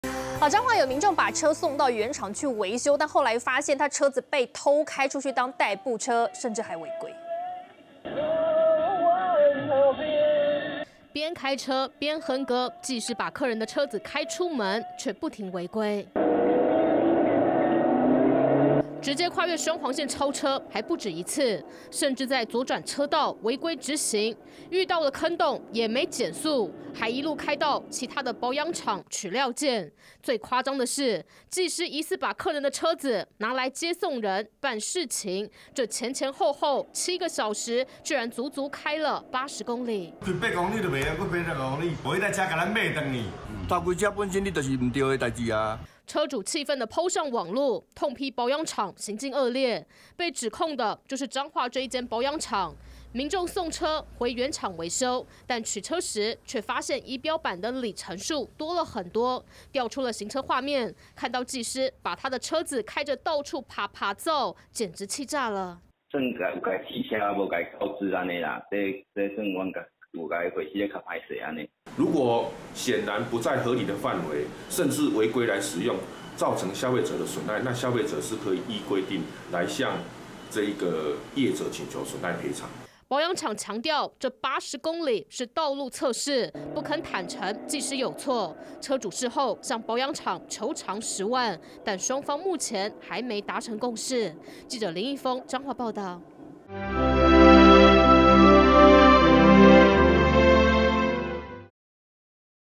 愛車送原廠維修！ 竟遭技師偷開80km「還違規」
彰化有民眾把車子送到原廠維修，但取車時發現儀表板里程數，多了80公里，調出行車畫面，看到車子被技師開出去辦事情，甚至到其他保養廠取料件，過程中還多次違規，氣的列出六大罪狀，控訴保養廠，但對方卻硬掰是「道路測試」，事後車主向業者索討10萬賠償費，但目前雙方還沒達成共識。

下載【年代電視】APP：
iOS平台：